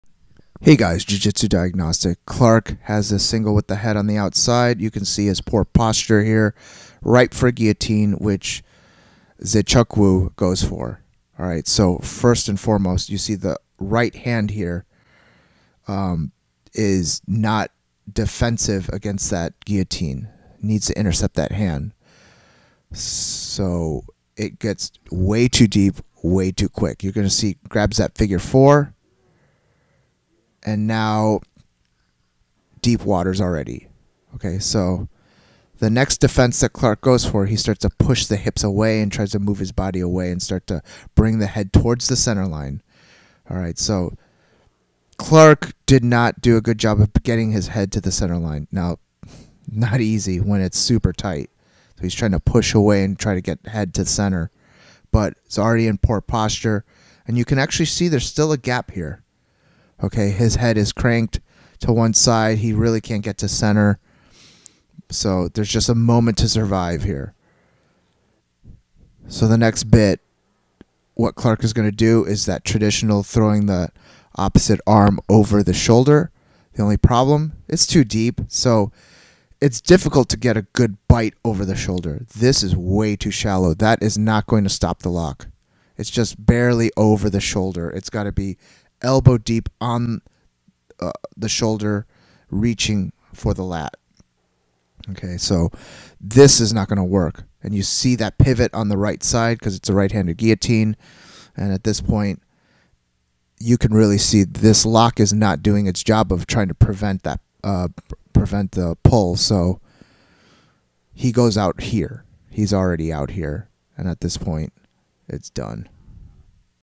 Kennedy Nzechukwu v. Devin Clark | Guillotine Choke COMMENTARY | UFC 288
Criticism and commentary of a short portion of the footage (protected under fair use) of what went wrong for Devin Clark's guillotine choke defense and counters and what he could have done to possibly change the outcome.